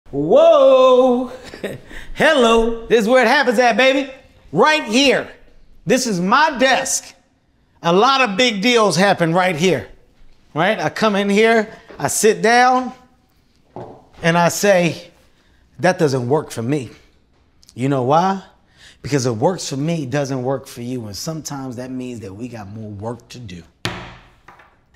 Bossman Kevin Hart
Kevin Hart enters boss mode when he sits at his swanky new desk!